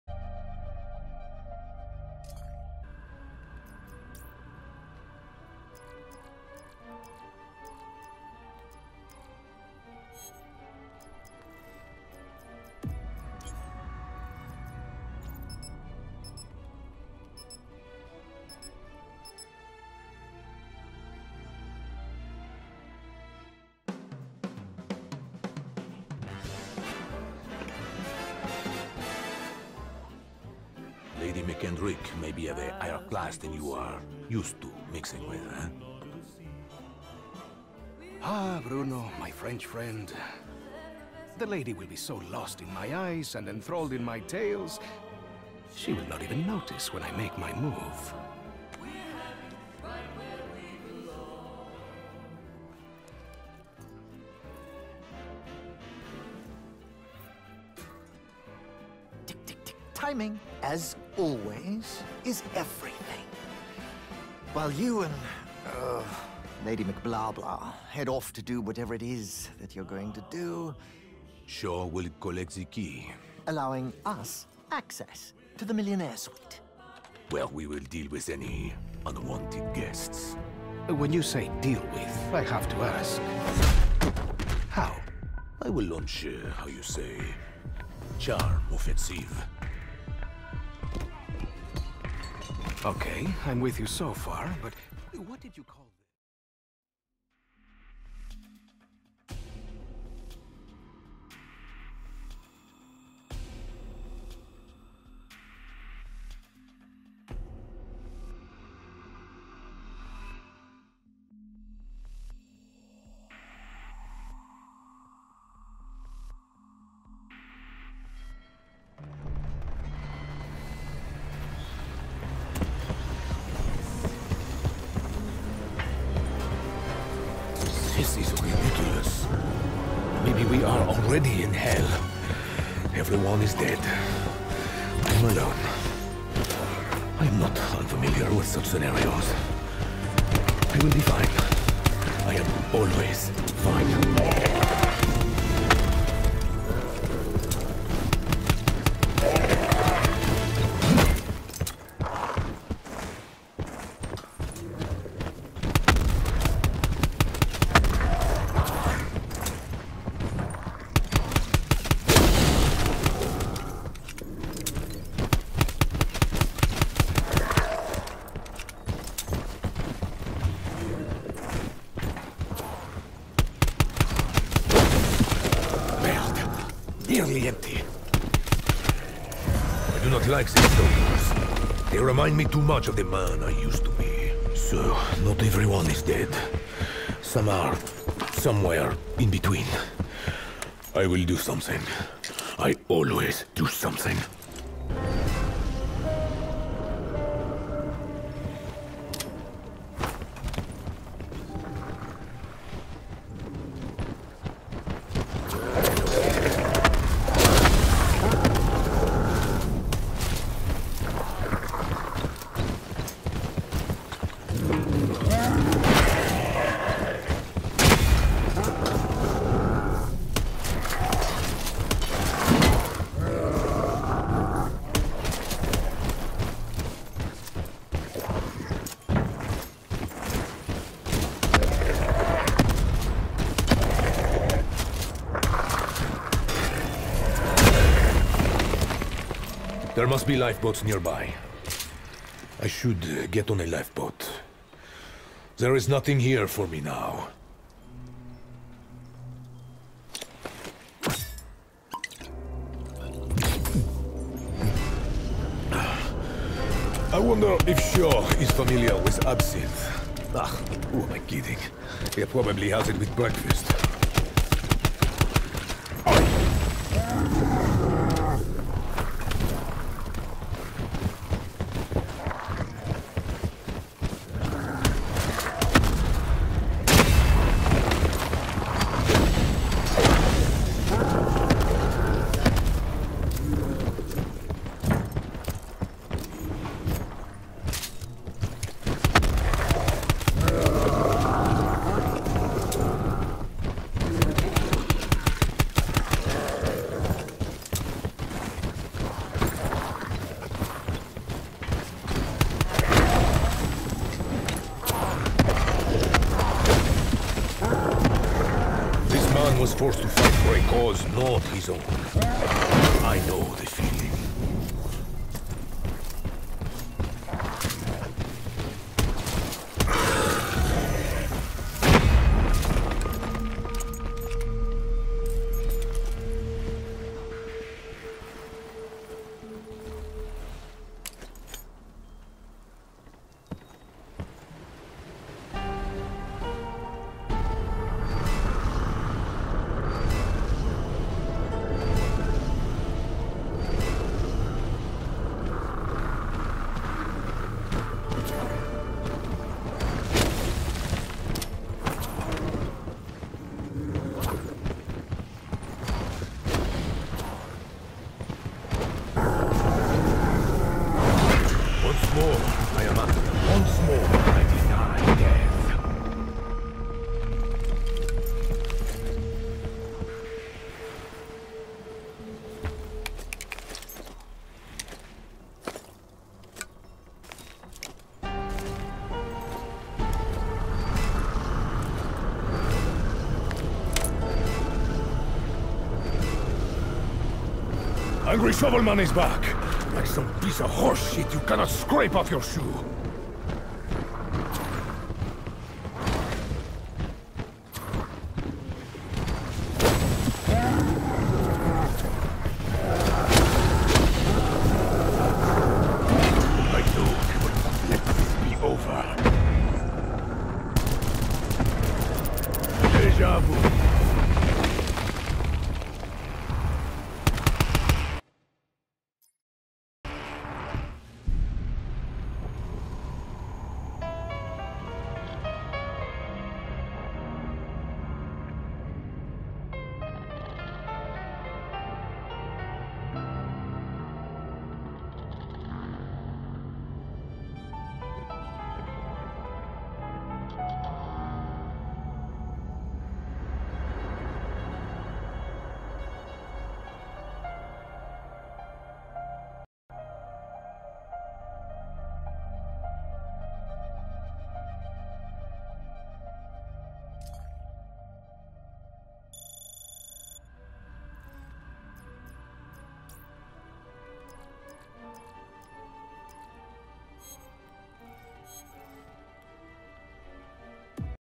Stoker Challenge NinjaNation Comp
In this video i do one of the challenges for the NinjaNation zombies tournament to gain season points for my team, team fury